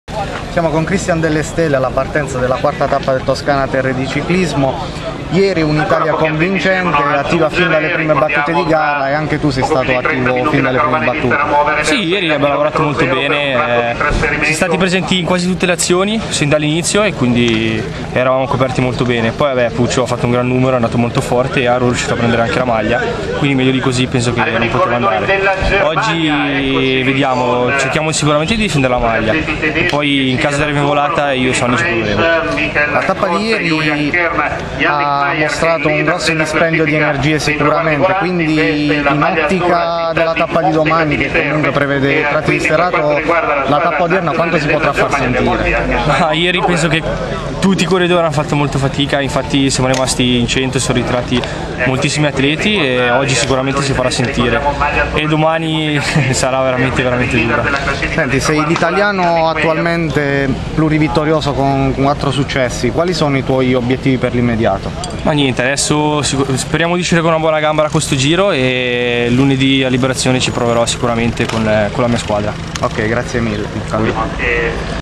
Toscana-Terra di Ciclismo - 4a tappa - Intervista a Christian Delle Stelle prima della partenza
Intervista a Christian Delle Stelle alla partenza della quarta tappa del Toscana-Terra di Ciclismo